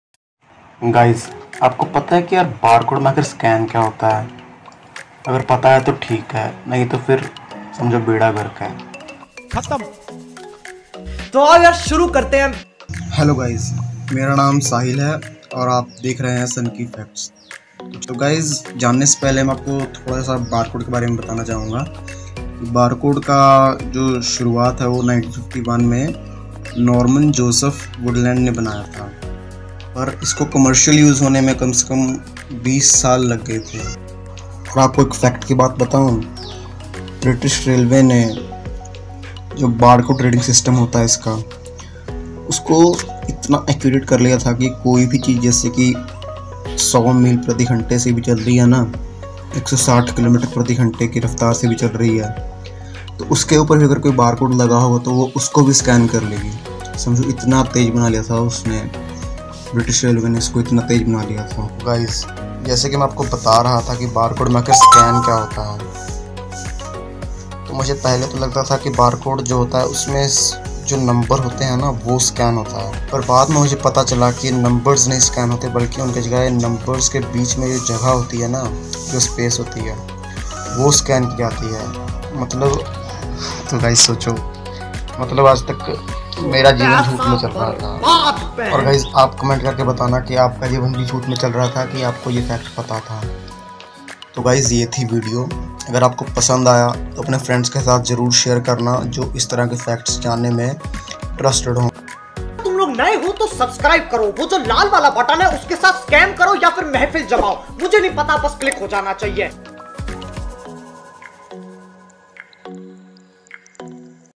आखिर बारकोड मैं क्या स्कैन होता है। fact about barcode
दोस्तो इस वीडियो मैं मैंने आपको बताया है की बारकोड मैं स्कैन क्या होता है।यकीन मानिए यह मुझे जानकर भी हैरानी हुई थी और अब मैं आपको हैरान करना चाहता हूं आप वीडियो देखे और कमेंट सेक्शन में बताए की वीडियो कैसी लगी अगर कोई प्रोब्लम है तो कमेंट मैं बता सकते है।
धन्यवाद।